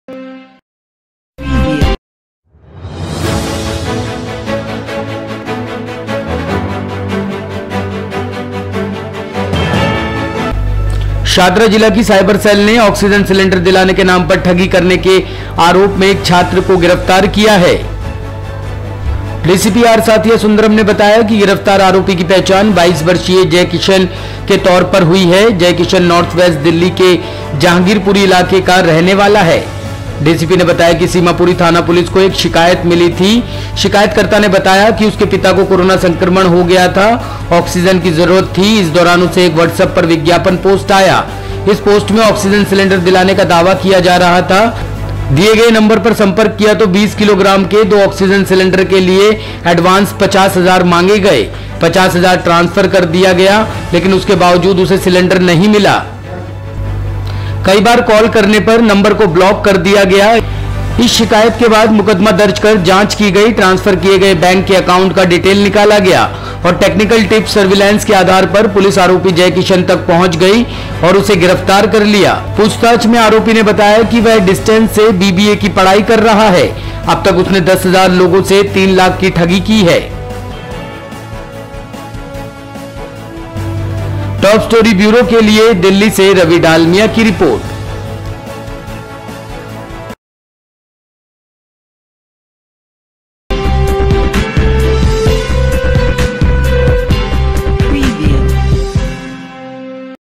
BBA का Student कर रहा था Oxygen सिलेंडर दिलाने के नाम पे ठगी,10 लोगो को लगाया 3 लाख का चुना
BBA का Student कर रहा था Oxygen सिलेंडर दिलाने के नाम पे ठगी,10 से ज्यादा लोगो को लगाया 3 लाख का चुना

शाहदरा जिला की सााइबर सेेेल ने ऑक्सीजन सिलेंडर दिलाने के नाम पर ठगी करने के आरोप में एक छात्र को गिरफ्तार किया है। डीसीपी आर साथिया सुंदरम ने बताया कि गिरफ्तार आरोपी की पहचान 22 वर्षीय जय किशन के तौर पर हुई है,जय किशन नॉर्थ वेस्ट दिल्ली के जहांगीरपुरी इलाके का रहने वाला है।